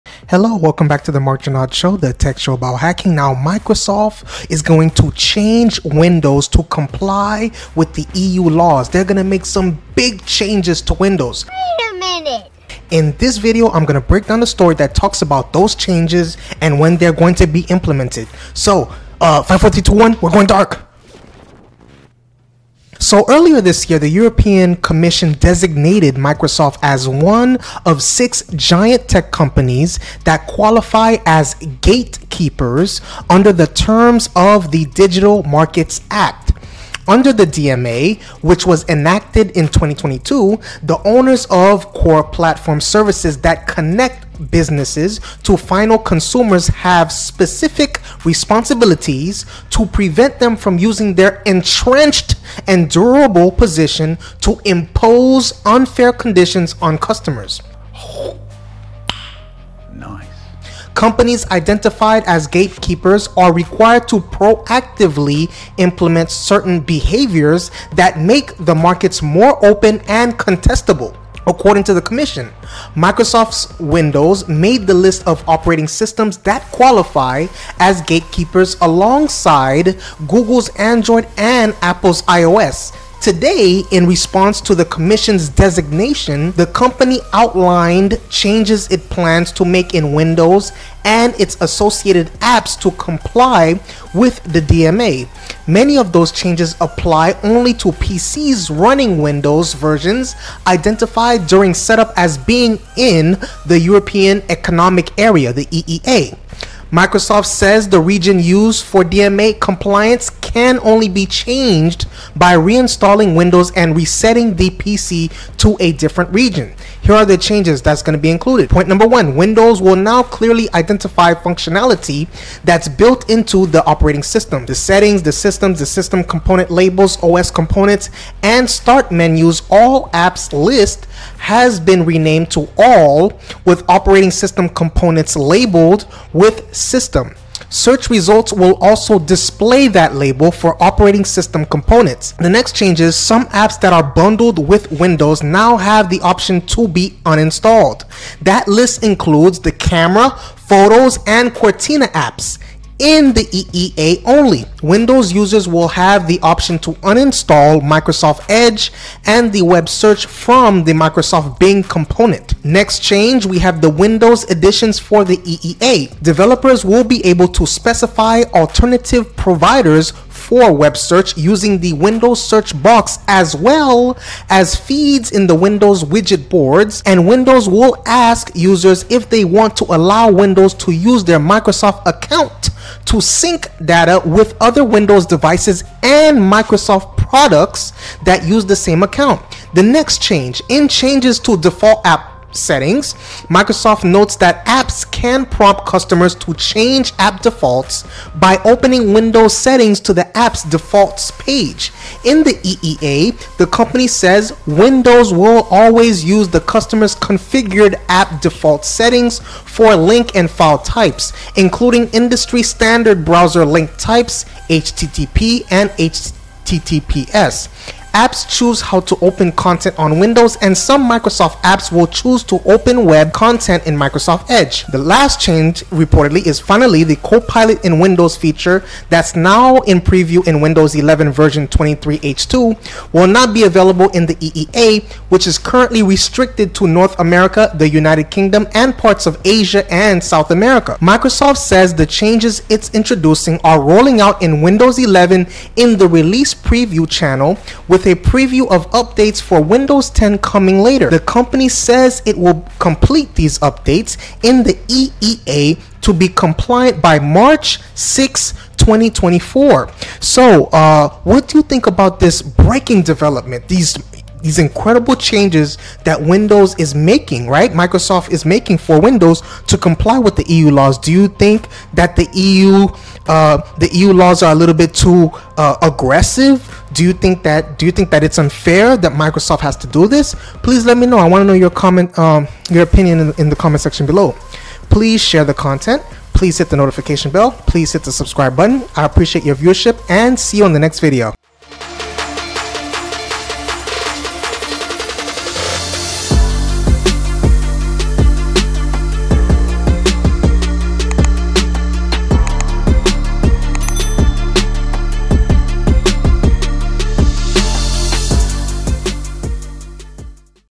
The EU Fixed Microsoft Windows
The European Union now requires that Microsoft Windows provide the ability to uninstall default applications to end users. The default applications in scope are Edge, Cortana, and Bing from Microsoft Windows 10 and Microsoft Windows 11. According the many reports, the new law that the EU has pushed is expected to take place around March 2024. In addition, apps that are bundled with Microsoft Windows, like Camera and Photos are now required to have an uninstall feature according to the EU. What do you think about the EU requiring changes from Microsoft Windows?